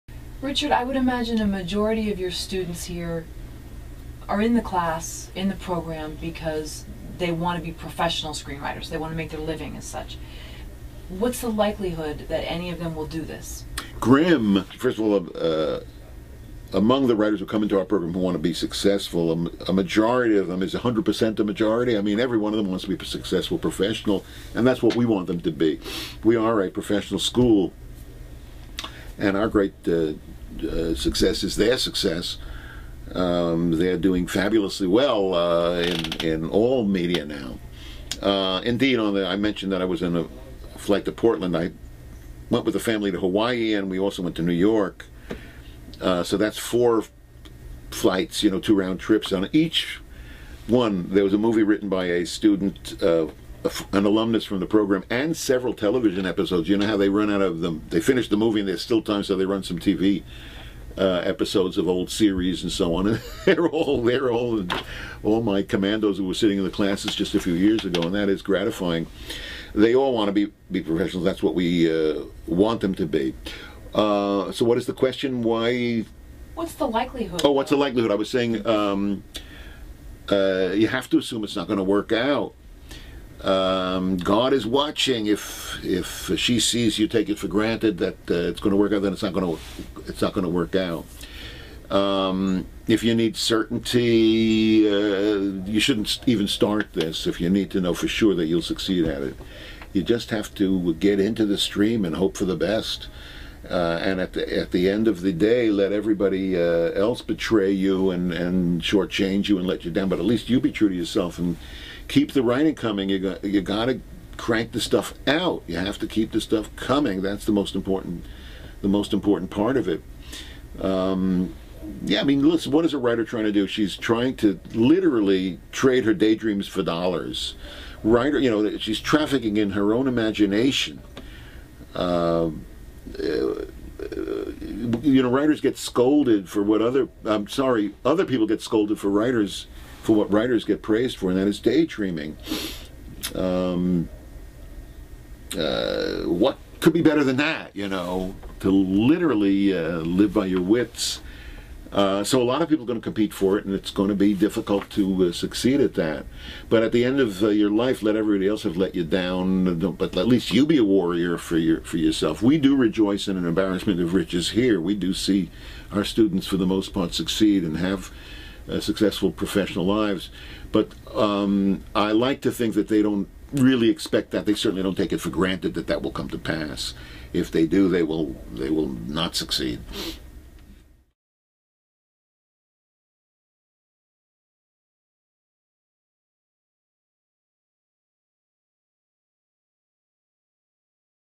What Is The Likelihood A Student Screenwriter Will Become A Professional? by Richard Walter
The Art, Craft, and Business Of Film and Television Writing

AUDIO